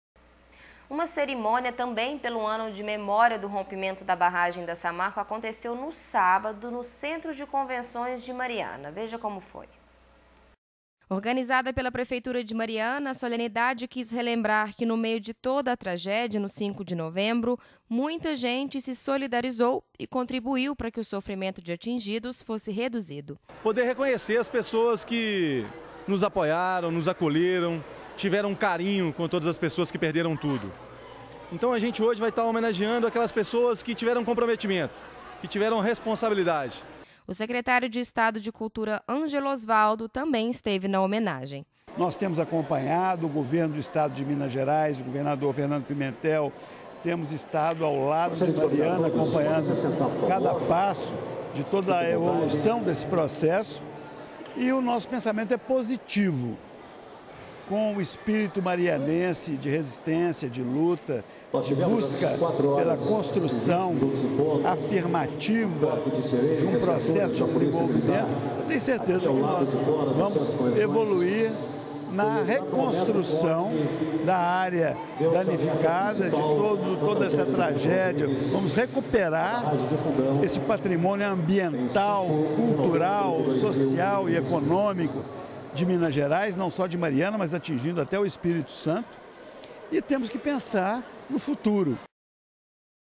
Cerimônia cívica em homenagem aos voluntários solidários pelas vítimas da barragem de Fundão
Uma cerimônia também pelo ano de memória do rompimento da barragem da Samarco aconteceu no dia 05 de novembro no centro de convenções de Mariana